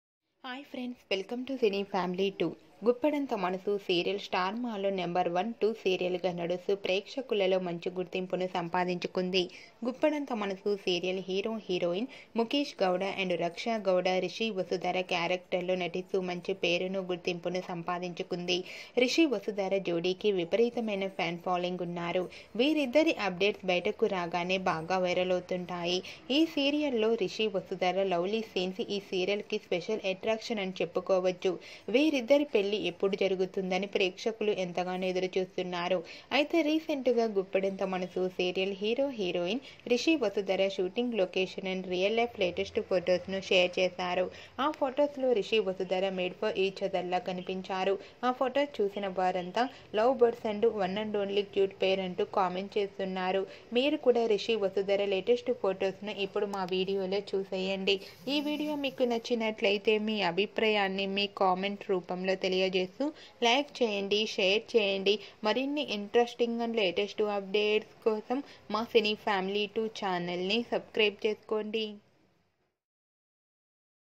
తన ఫ్రెండ్ వెడ్డింగ్ కి సందడి చేసిన రిషి
guppedanthamanasu serial hero and heroine rishi vasudhara shooting location latest photos
guppedanthamanasu serial fame rishi real life latest photos